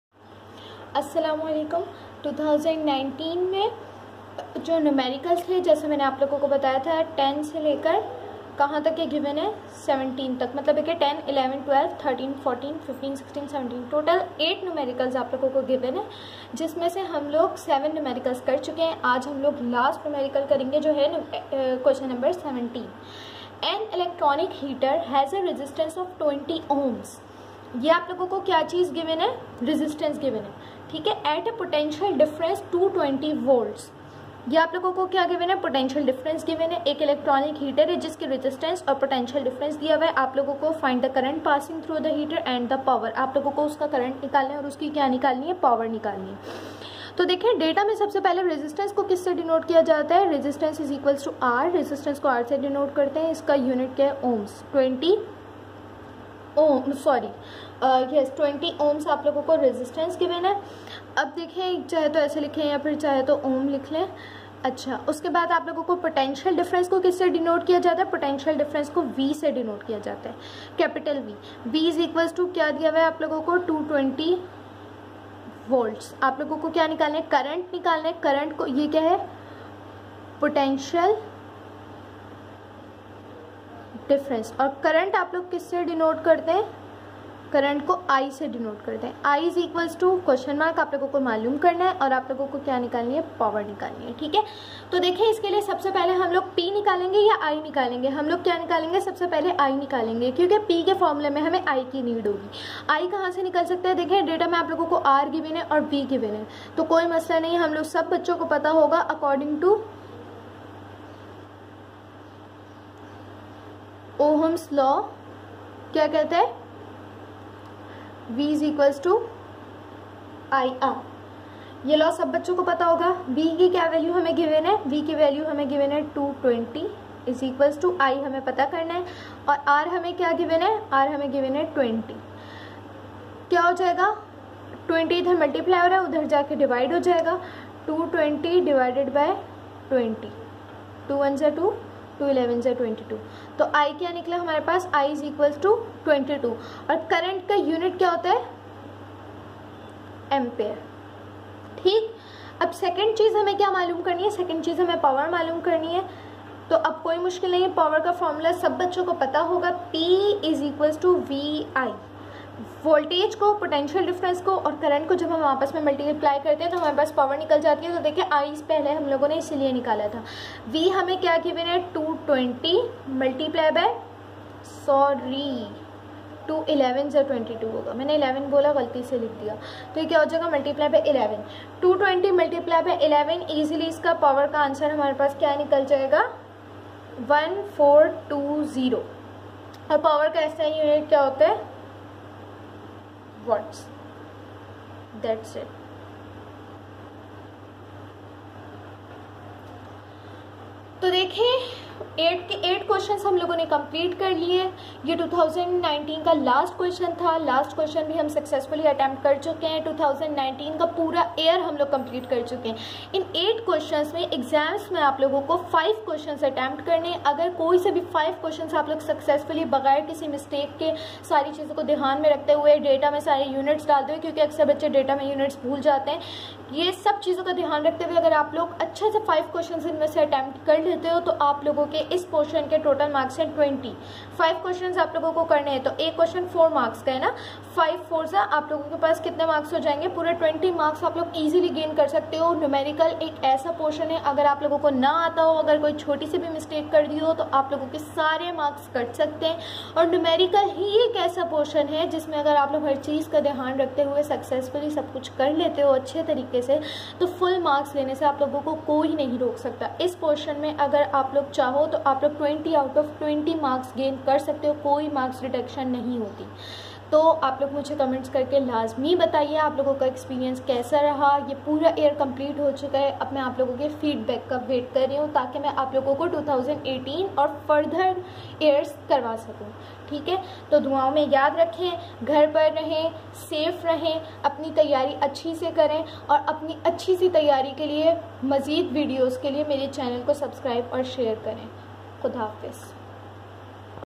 X-Physics, 10 years solved numericals, 2019, Q#17
Past years solved numericals of physics for matric students.
For the preparation of exams,